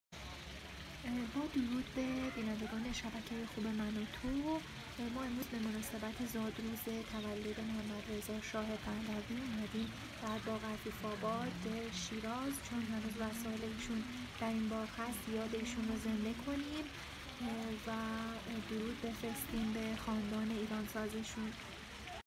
به مناسبت زادروز محمدرضا شاه پهلوی
بازدید گزارشگر منوتو از باغ عفیف‌آباد در شیراز به مناسبت زادروز محمدرضا شاه